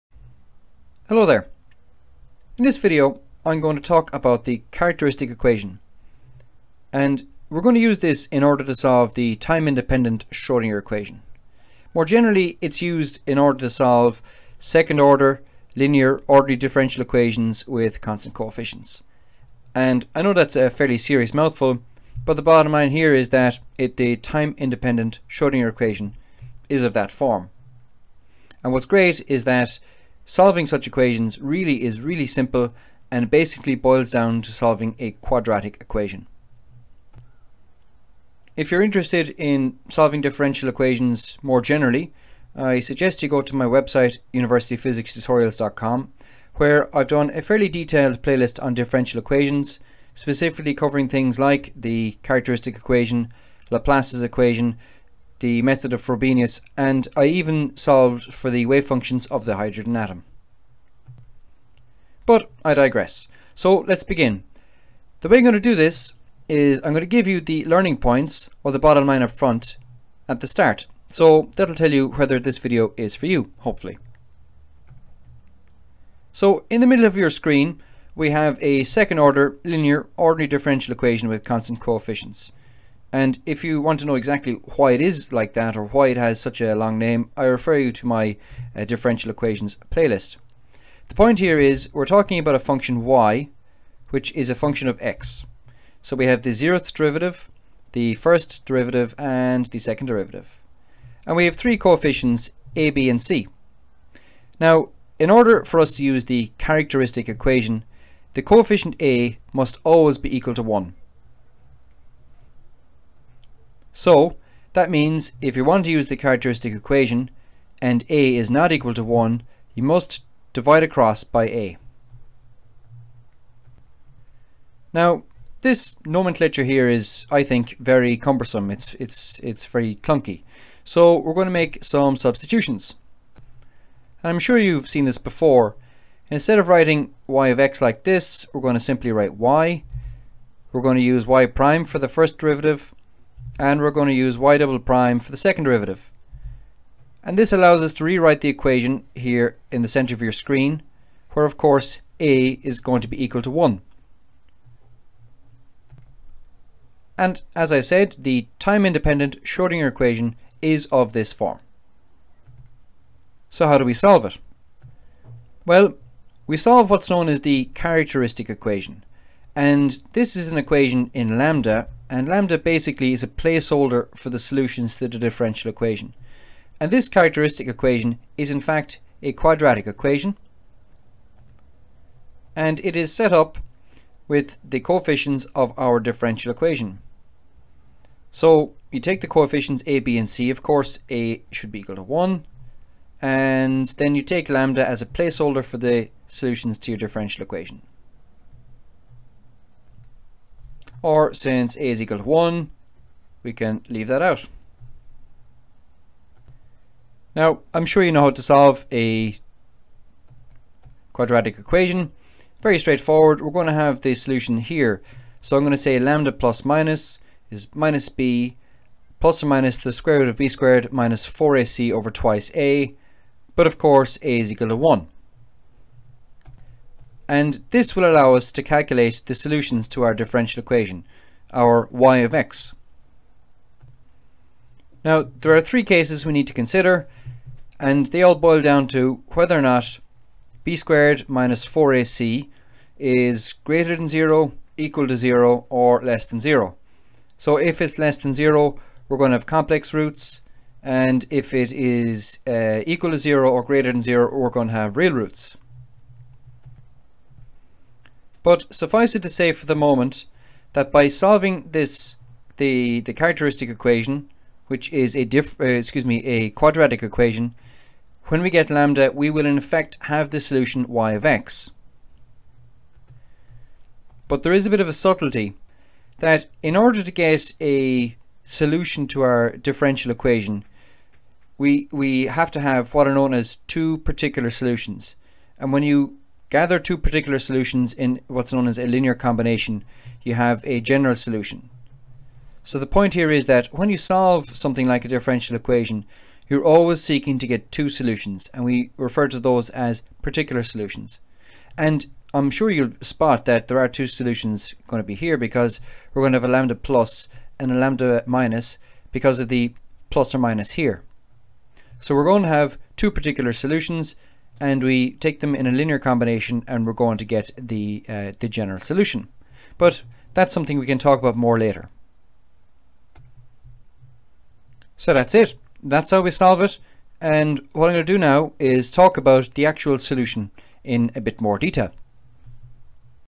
Characteristic Equation 1: Learning Points
Topics covered in this video segment.

The characteristic equation

Real and complex exponential (sinusoidal) solutions to the differential equation